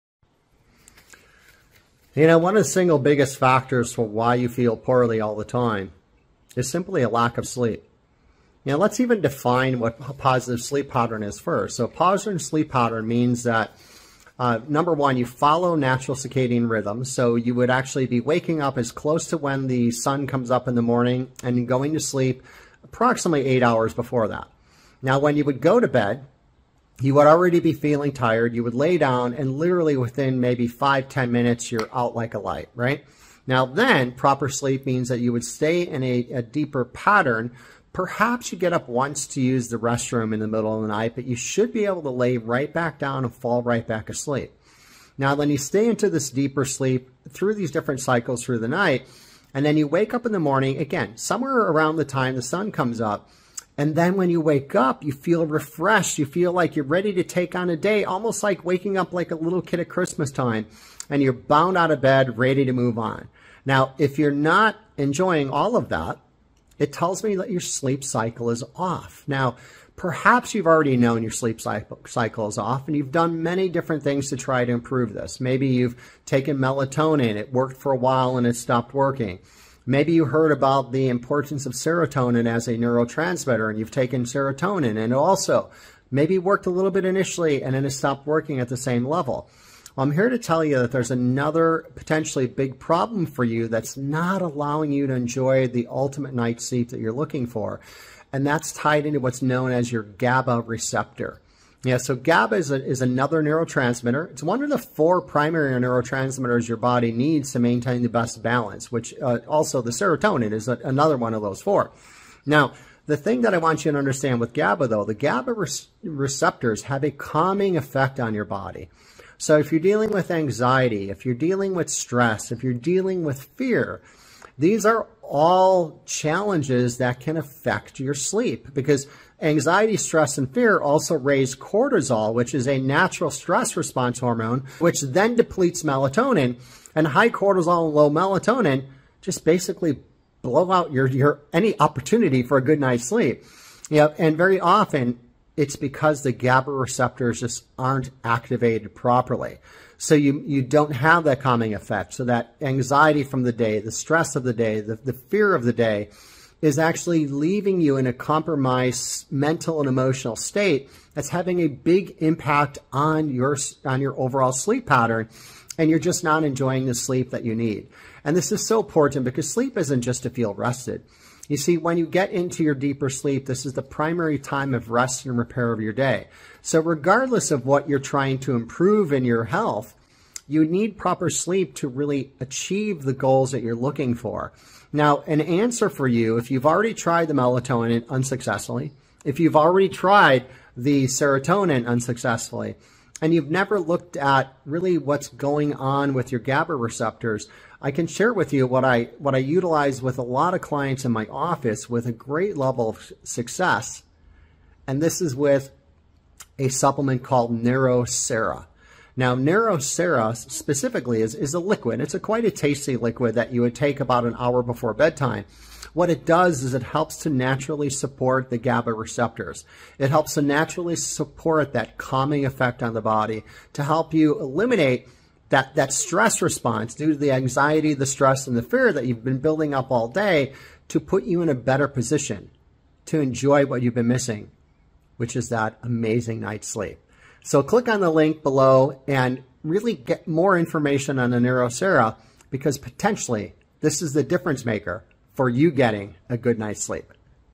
Unlock the Power of GABA Receptions and Sleep Tonight | Depke Wellness
Learn how you can help your body to naturally achieve an improved night's sleep. Insomnia and overall poor sleep patterns can leave you fatigued, dealing with brain fog, drowsiness, irritability, headaches and a constant need for caffeine.

To get the cutting edge holistic health information you're looking for, be sure to follow this channel and click the bell on the upper right, so you get everything you're looking for.